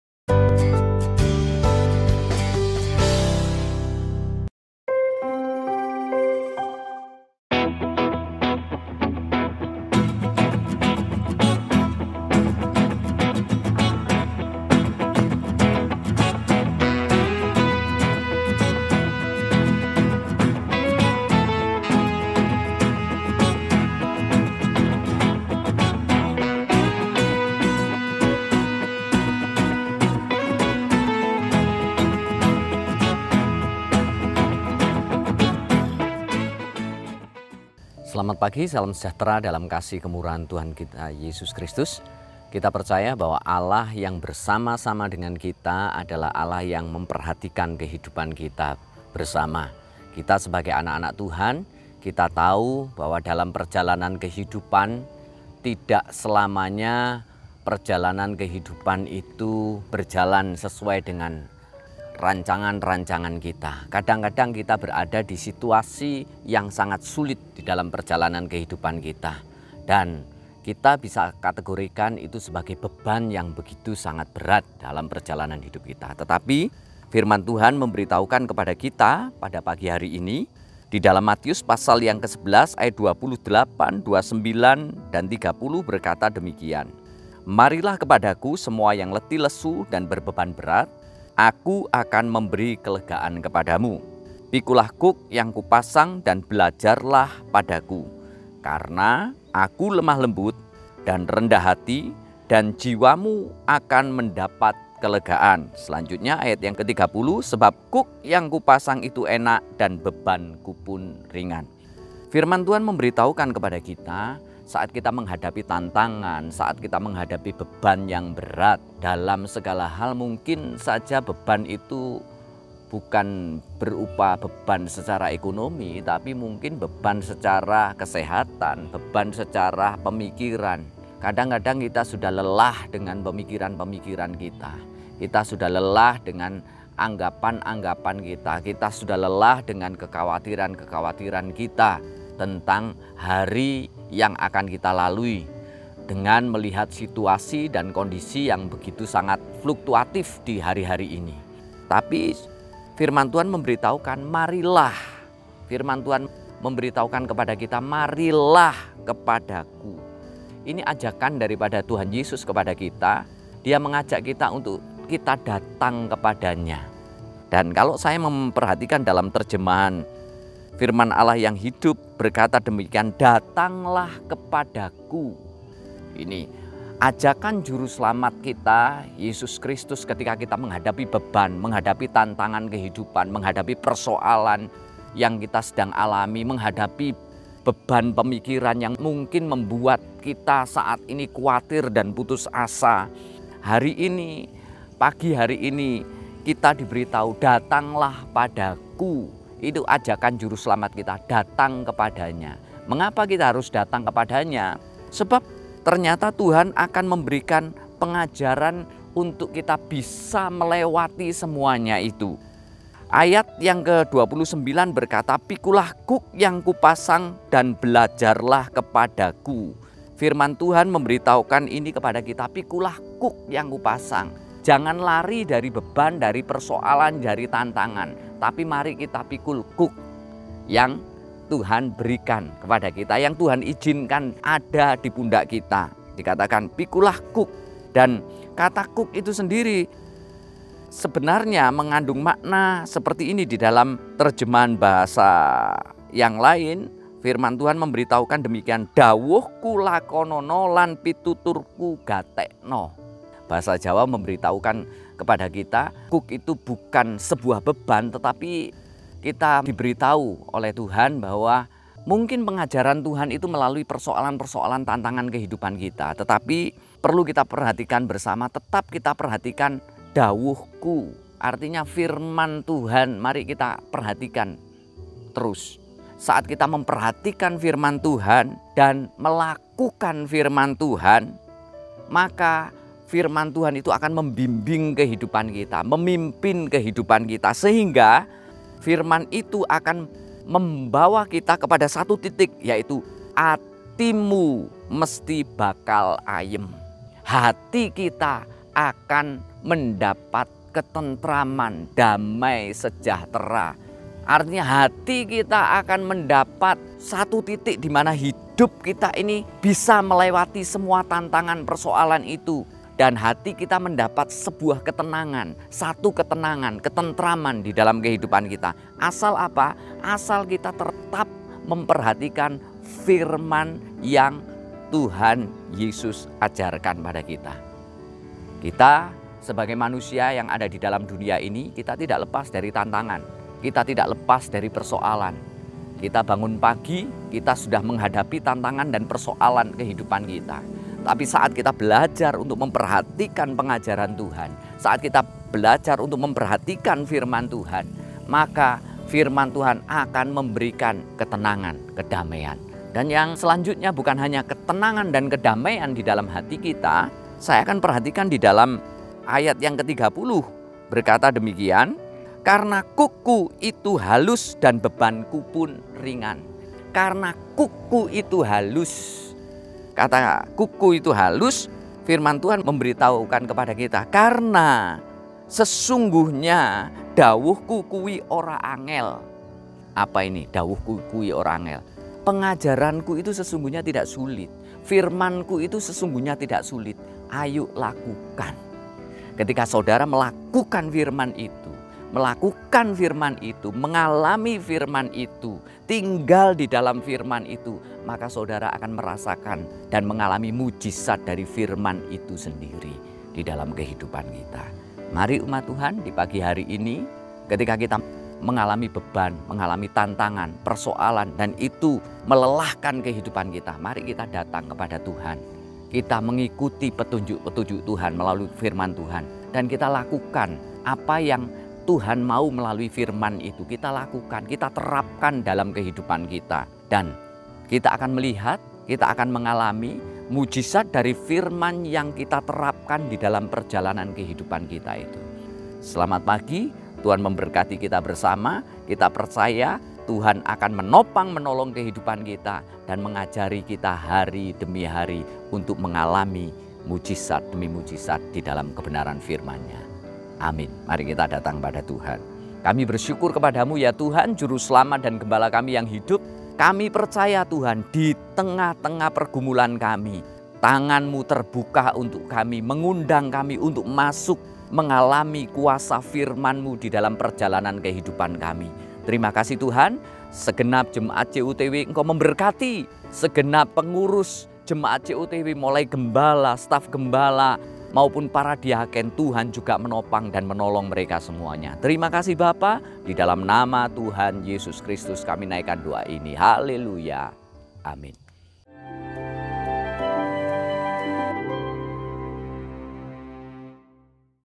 MENGALAMI KUASA FIRMAN TUHAN, Renungan Pagi 1 Mei 2024
Pdt. Bambang Tri Susilo

Lokasi: Rawaseneng, Temanggung